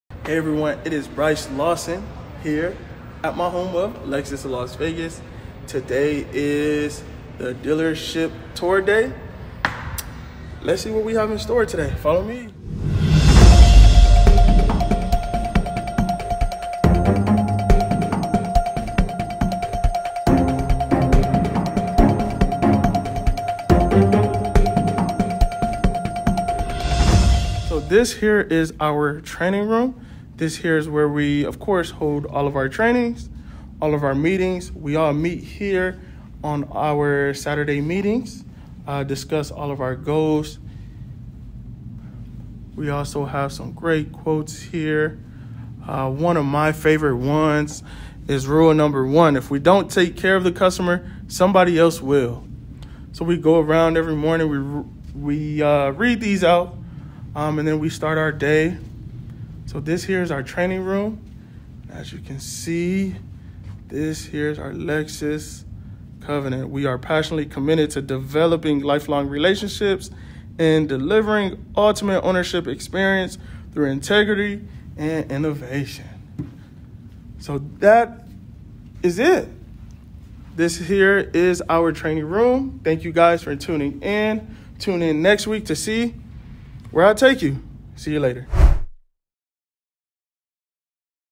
Dealership tour-Lexus of Las Vegas-Meeting room and virtual tour
Bryce Lawson takes you behind the scenes at Lexus of Las Vegas. This week he shows us the Meeting room. Here is a link to our dealership virtual tour!